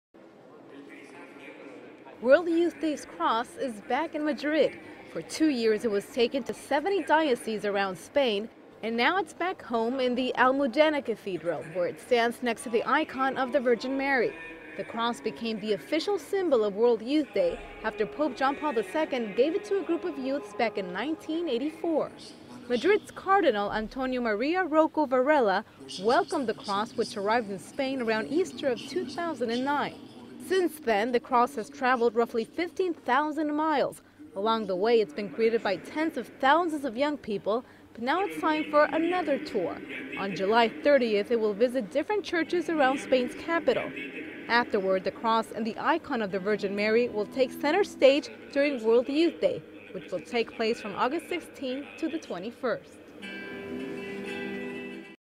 World Youth Day's Cross is back in Madrid
July 20, 2011. (Romereports.com) For two years it was taken to 70 dioceses around Spain. Now it's back home in the Almudena Cathedral, where it stands next to the icon of the Virgin Mary. The cross became the official symbol of World Youth Day, after pope John Paul II gave it to a group of youths back in 1984.

Madrid's Cardinal Antonio Maria Rouco Varela welcomed the Cross, which arrived in Spain around Easter of 2009.

Since then the cross has traveled roughly 15,000 miles. Along the way its been greeted by tens of thousands of young people. Now it's time for another tour. On July 30th, it will visit different churches around Spain's capital.

Afterward, the cross and the icon of the Virgin Mary will take center stage during World Youth Day, which will take place from August 16th to the 21st.